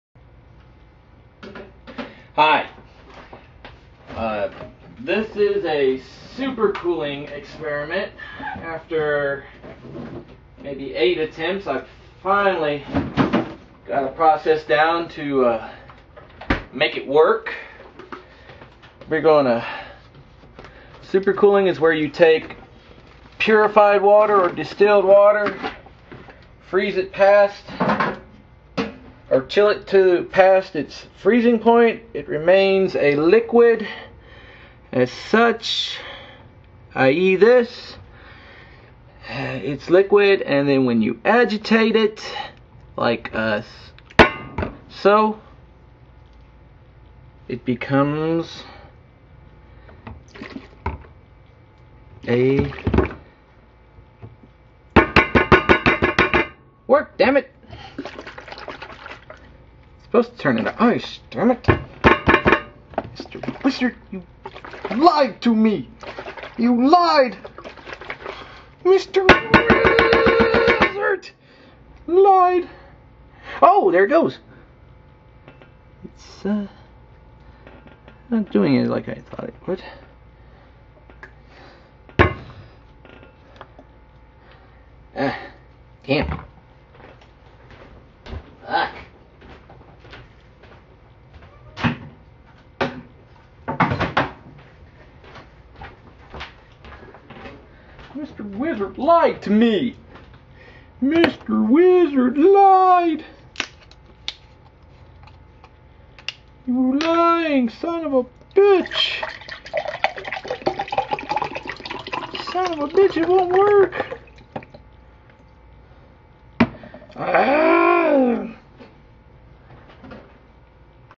Supercooling Experiment gone wrong.
This was an earlier attempt at supercooling. The second one, actually. It did not turn out like it was suppose to. Hilarity ensues.